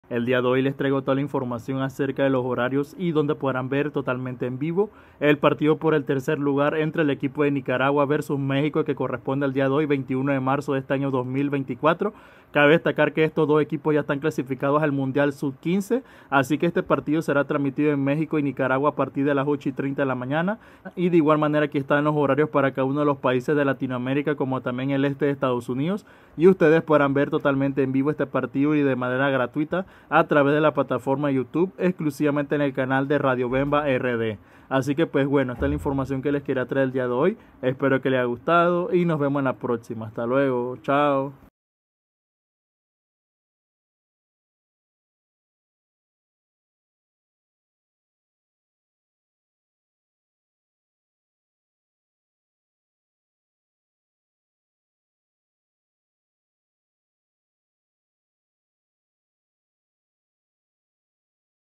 ⚾️Nicaragua vs México⚾️Dónde Ver El Partido EN VIVO|Tercer Lugar del Premundial U15 de Béisbol 2024
En este video les estaré mostrando todo acerca de los horarios como tambien donde podrán ver el partido de Nicaragua vs México por el tercer lugar en el Premundial U15 de Béisbol 2024.